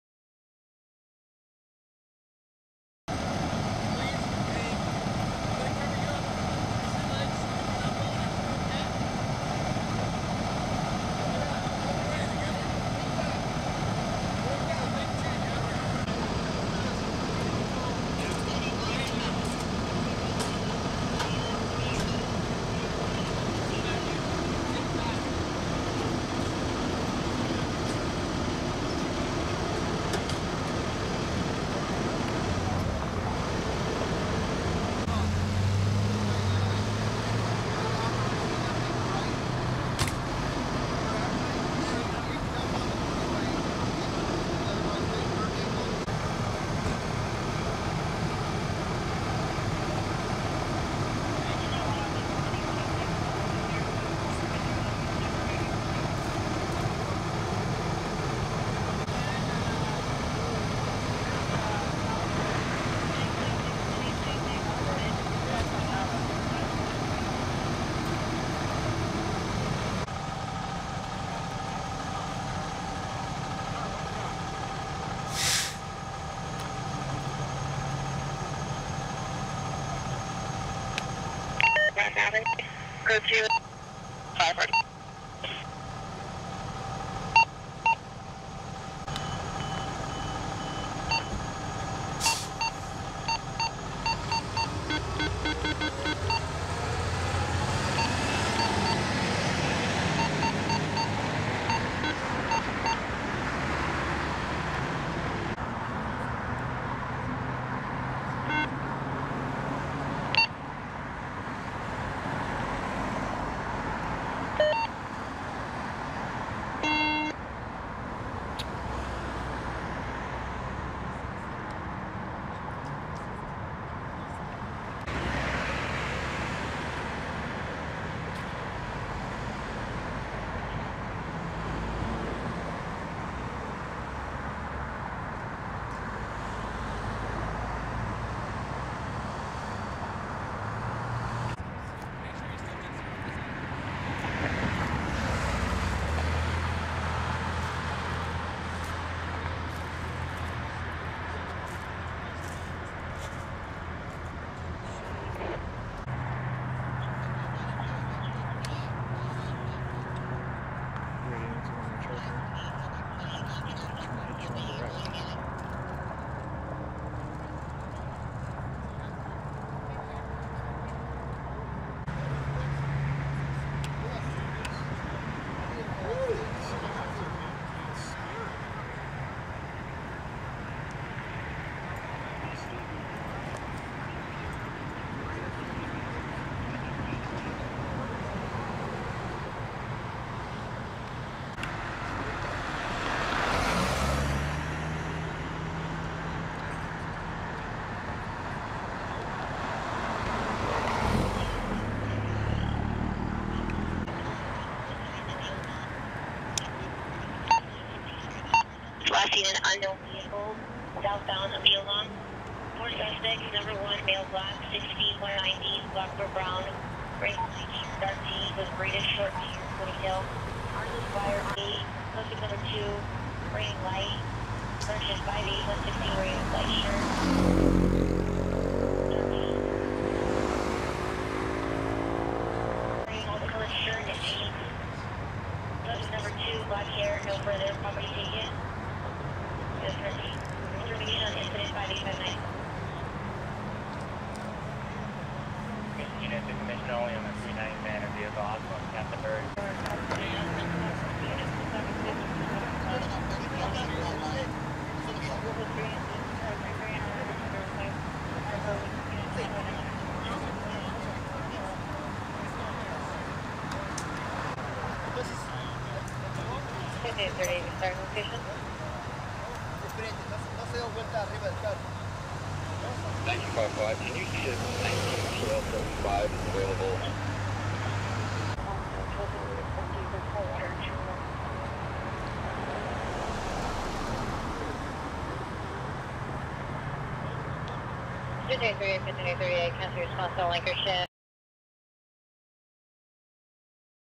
Auto vs Pedestrian / Encino RAW FOOTAGE
2-1-18

ENCINO - At approximately 2138, LAFD and LAPD responded to reports of an auto vs pedestrian accident on Burbank  Blvd, near Woodley Ave. Arriving units found a male victim who had been struck by a vehicle. The vehicle was about 1/8 of a mile down the road. There was moderate damage to the vehicle. The victim was transported to the hospital in unknown condition. The cause of the collision is unknown. VTD officers were still at the scene as of 0030. LAPD Valley Traffic is handling the investigation of the accident.

During the response, on the LAPD radio, a stabbing suspect description was given for the location of the TC and an LAPD airship was above the scene, with its spotlight over the area. As well, a large amount of LAPD officers were on the scene with the TC. It is unknown if the incidents are related.